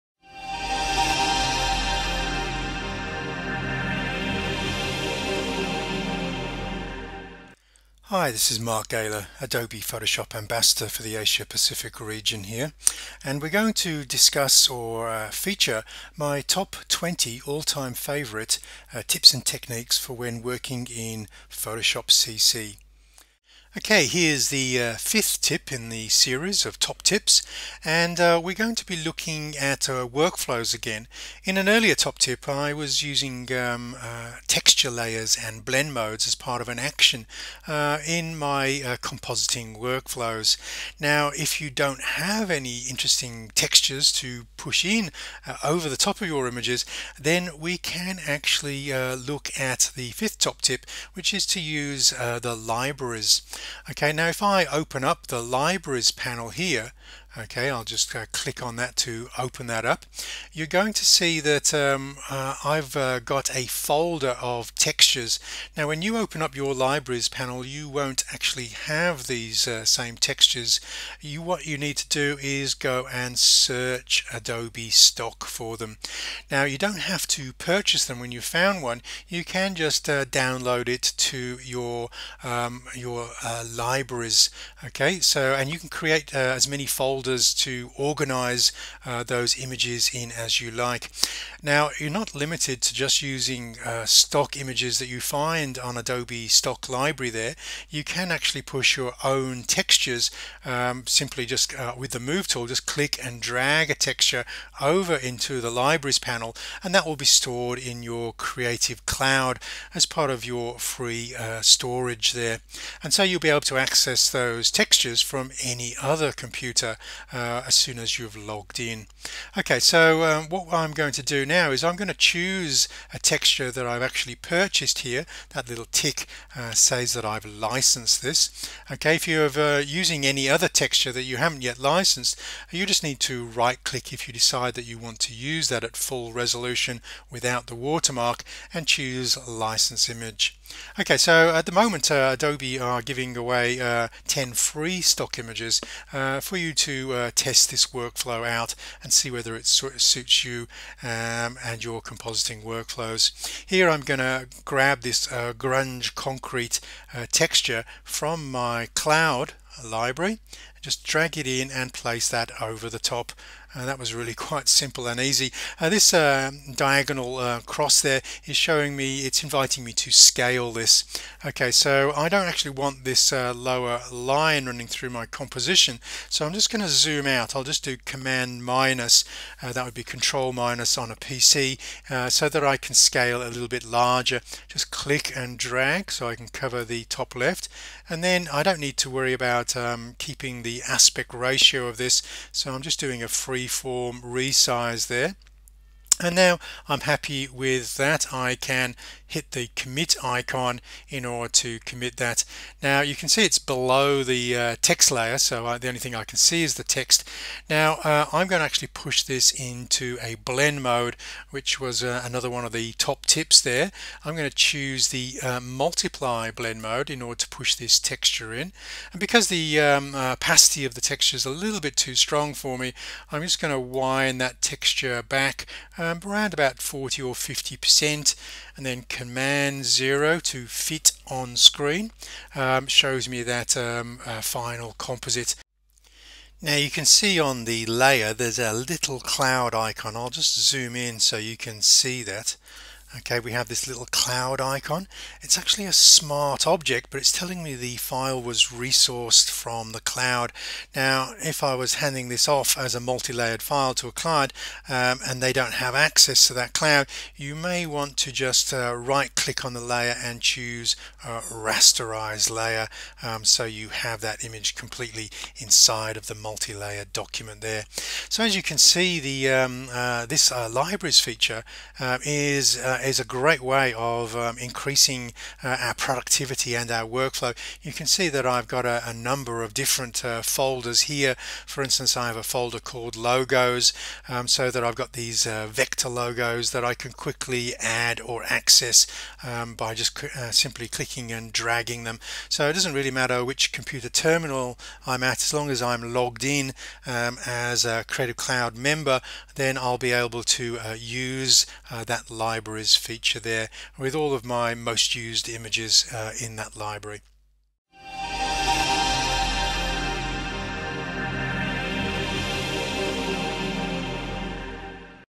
Top Tip 5 - Photoshop CC Top 20 Tips & Techniques - Libraries
Libraries
Photoshop CC Top 20 Tips & Techniques

Mark' Galer's Top Twenty Tips and Techniques to help you master Photoshop CC
Learn how to edit and process your photos with confidence using simple and quick processing techniques.

This series of tutorial movies have been created to help educate photographers how to organise (Catalog), select and process (Develop) their photographic images when using Adobe Photo Editing post-processing software.

Mark Galer is a photographer, educator and Sony Imaging Ambassador.